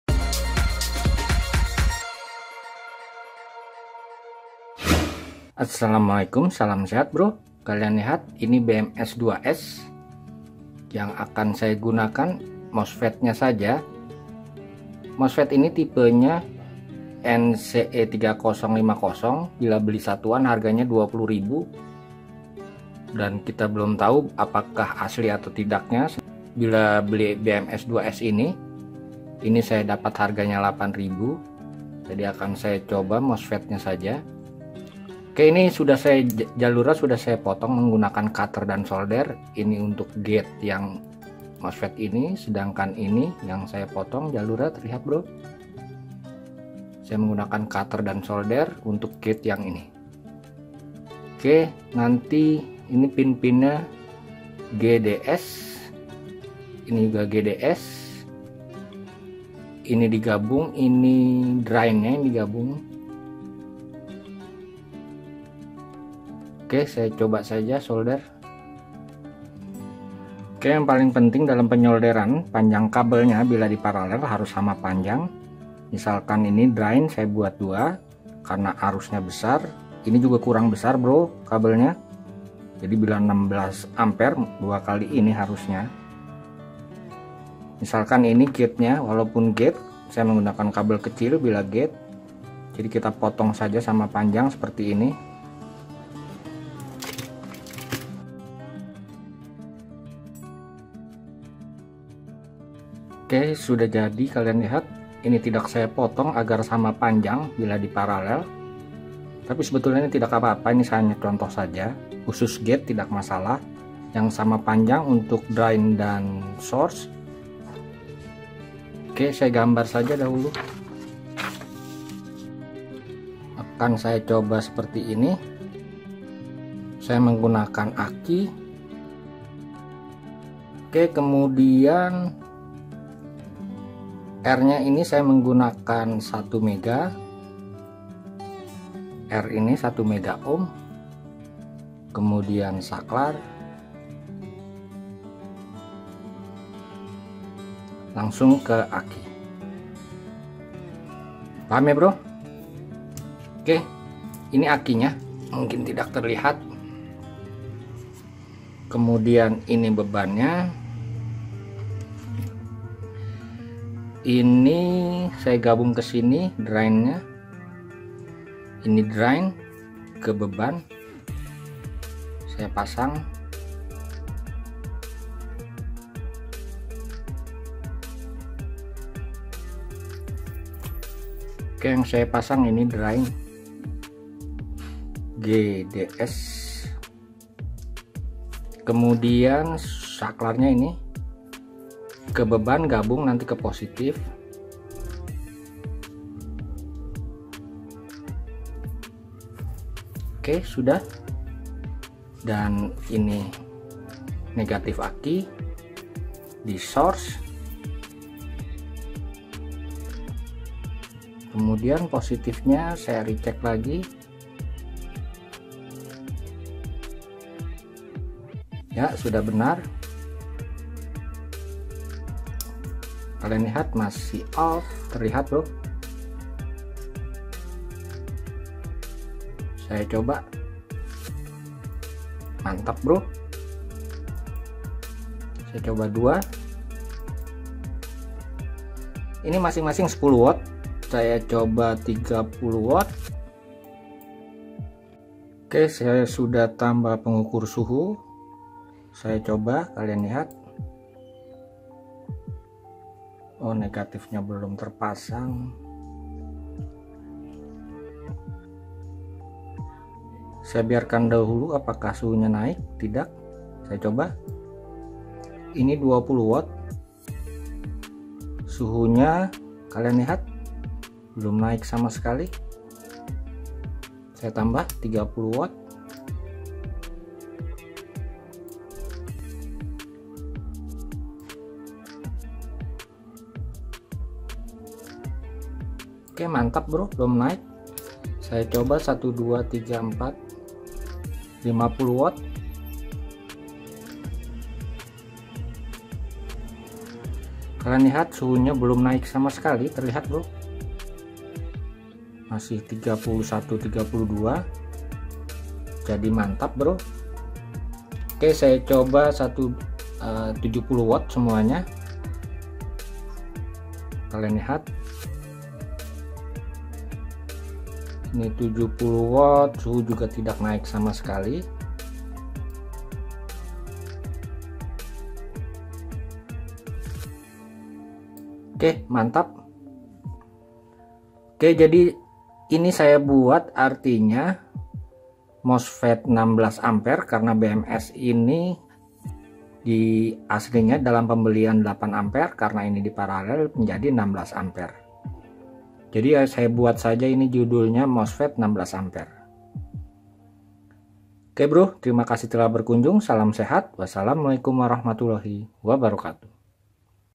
Relay saklar mosfet 16A dari BMS 2S 8A - Switch Mosfet NCE3050
Video ini menunjukan cara membuat Relay saklar mosfet 16A dari BMS 2S 8A - Relay Mosfet - Saklar mosfet, dengan menggunakan mosfet BMS.

00:00 Radal05
00:12 BMS 2S
00:48 Pin-pin mosfet NCE3050
01:00 Assembly
02:50 Test Saklar mosfet